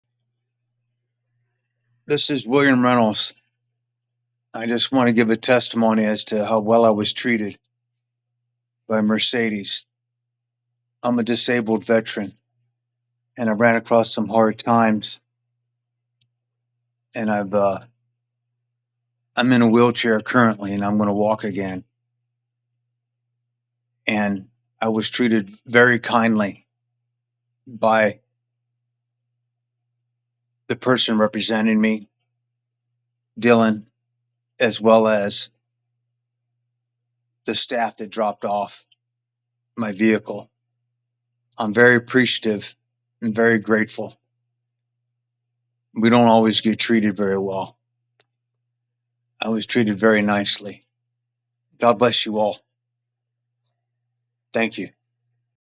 ⭐⭐⭐⭐⭐Customer Testimonial by William Reynolds For Mercedes-Benz of Jacksonville
🌟 A heartfelt thank you to William Reynolds, a respected disabled Veteran, for sharing his positive experience with Mercedes-Benz of Jacksonville. "I'm a disabled Veteran...I was treated very kindly..." - William Reynolds. 🇺🇸 Your service to our country is immensely appreciated & we salute you, William.

It's our honor to serve those who serve and who have served our country. Watch William's testimonial to hear his full story and discover the respectful, caring service that awaits every customer at Mercedes-Benz of Jacksonville.